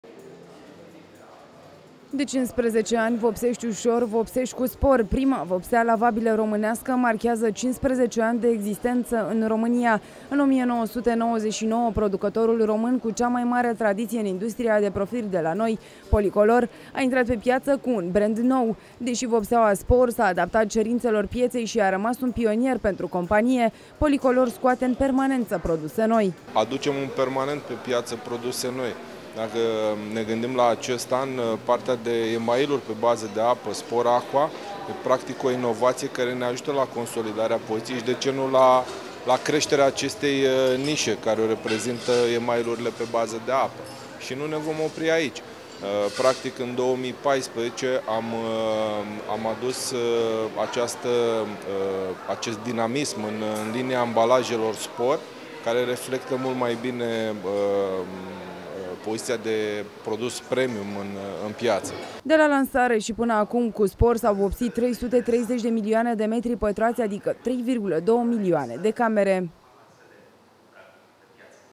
Conferinta SPOR 15 ani
3,2 milioane de camere vopsite cu lavabilă, 4,5 milioane de uși vopsite cu email, jumătate de milion de seturi de mobilier de grădină lăcuit, sunt doar câteva cifre din bilanțul unui brand care a scris istorie pe piața vopselelor din România: SPOR.